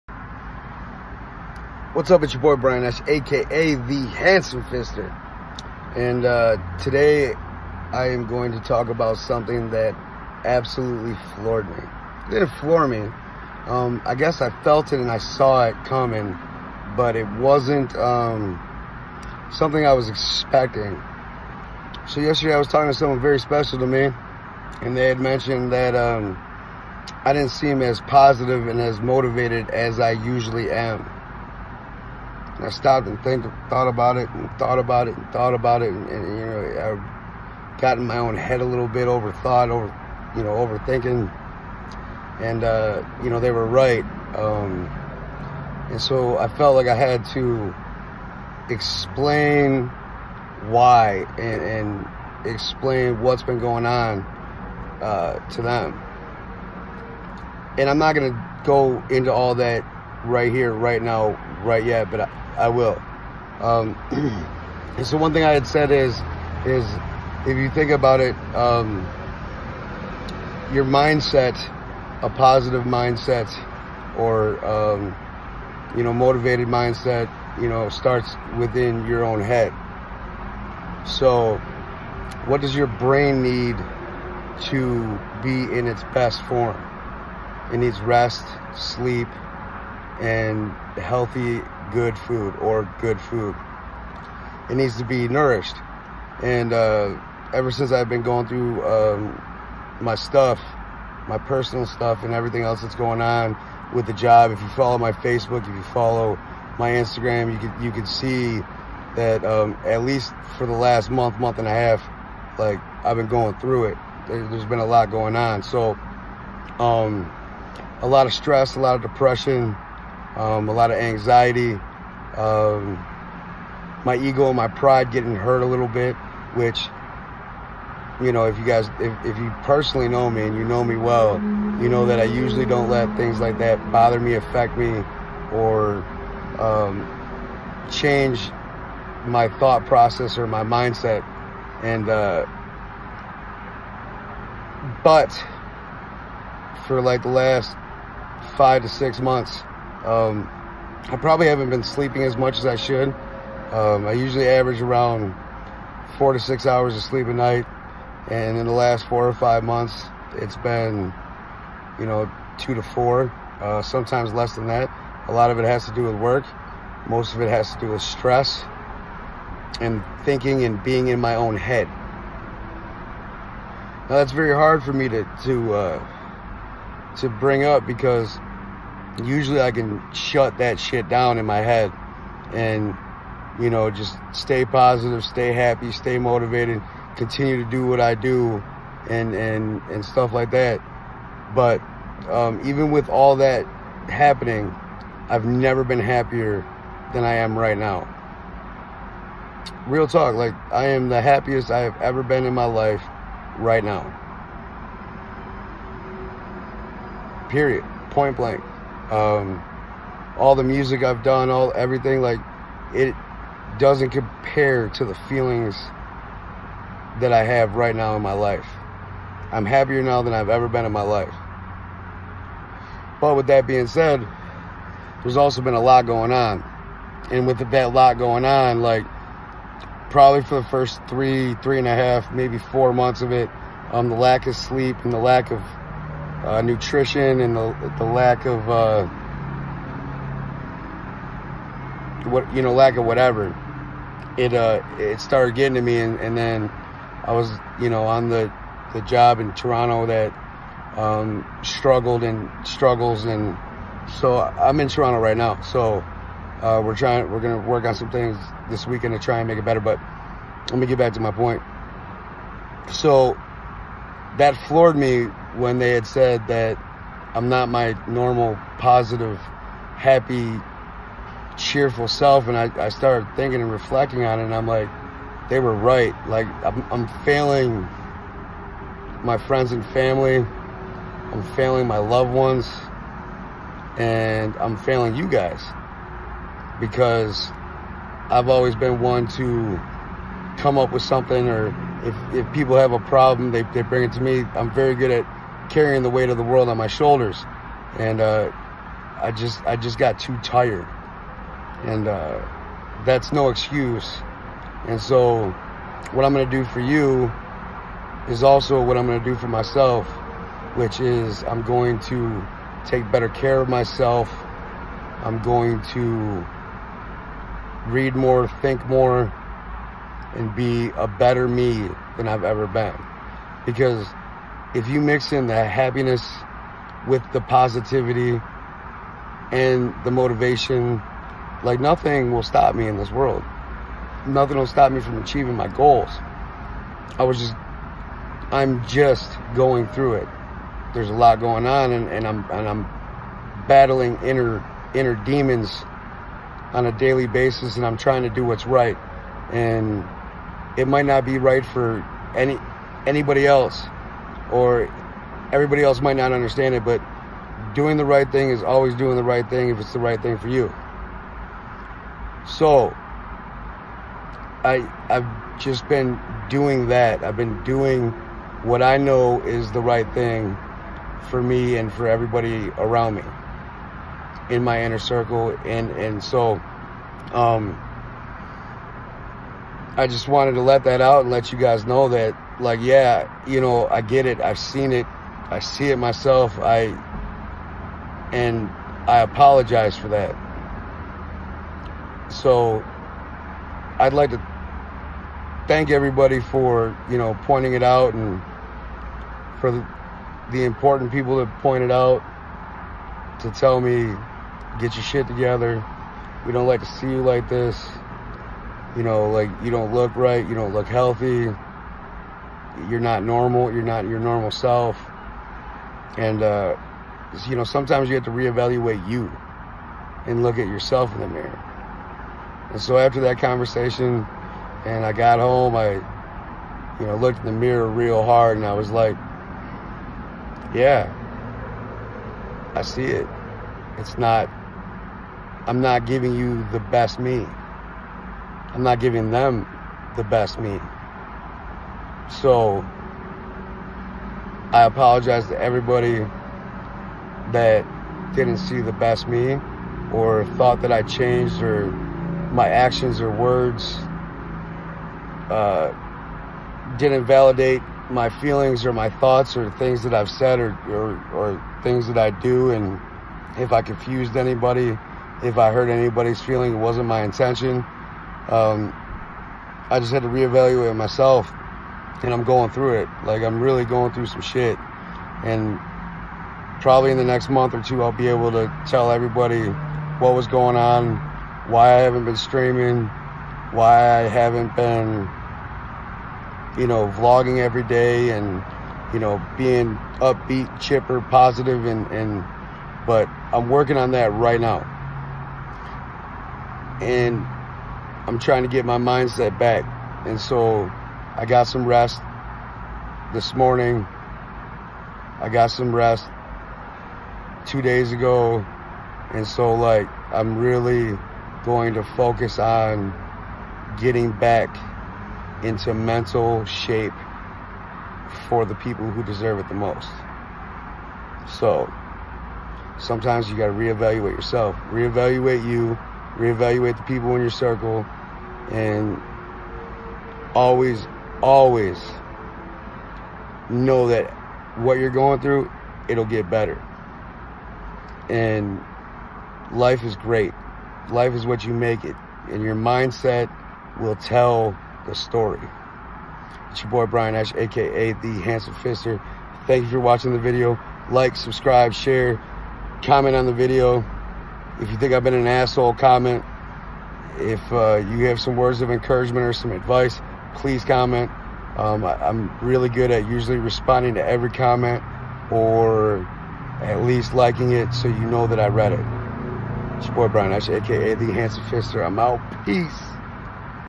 Words With Fister- Self Reflection
Someone very special and important to me pointed out that I have not been myself lately. I haven’t been as positive or motivated as usual. I explained, they completely understood but they were right! I’m going to fix it!